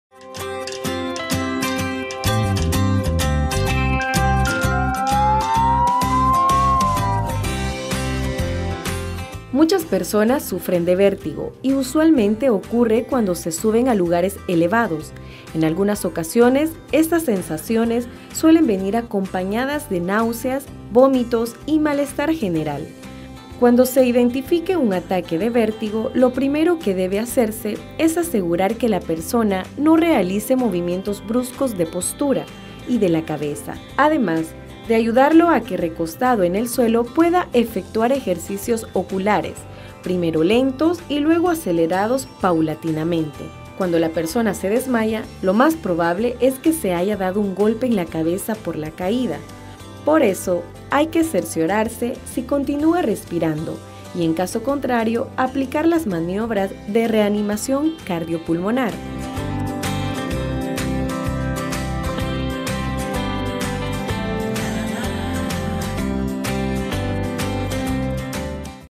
Primeros Auxilios
¿Cómo actuar ante un ataque de vértigo?